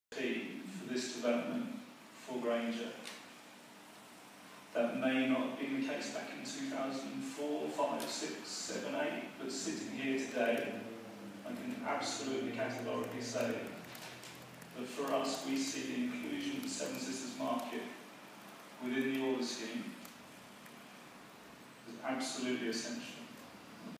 Grainger plc have come to love Tottenham's Seven Sisters indoor market
Grainger say inclusion of Seven Sisters indoor market is absolutely essential to Tottenham regeneration scheme. But admits it might not have always been their view. Jonathan Kiddle evidence on 12th July 2017 to Ward's Corner CZpO Public Inquiry.